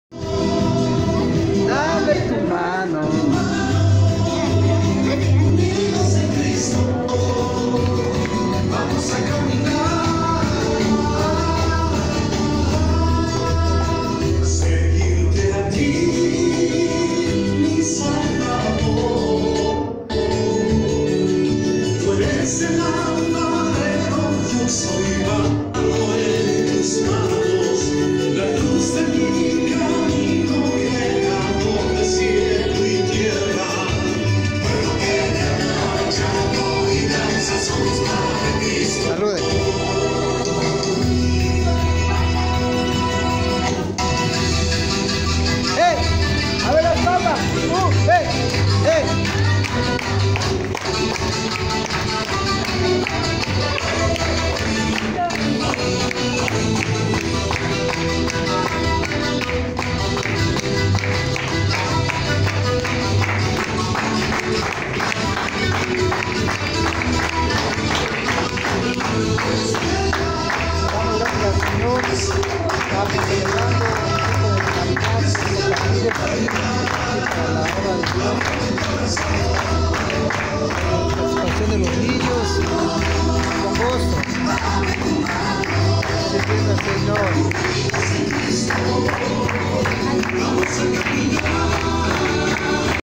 CAPELLANÍA MUNDIAL, NIÑOS DE ECUADOR DANZANDO PARA DIOS
NIÑOS DE CIELOS ABIERTOS QUITO, CELEBRAN AL SEÑOR JESUCRISTO COMO UNA OFRENDA DE AMOR Y ALABANZA.
CAPELLANÍA MUNDIAL DE PAZ LEVANTA BANDERA PROMOVIENDO EL AMOR DE DIOS EN LOS NIÑOS Y NIÑAS.